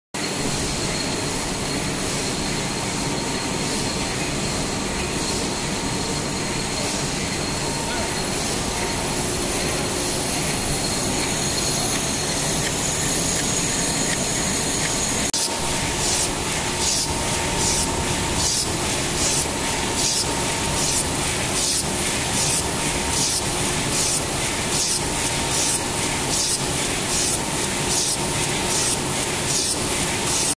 Egg viol label unit
It is a machine that is a continuation of the machines we make. This machine is the labeling machine that prepares the egg trays, which are finally finished, for sale. We can produce the machine you want according to the desired dimensions and plant work volume.

Address: Bozburun Mah. Nazif Zorlu Sit. 7156 Sk. No:12/A Denizli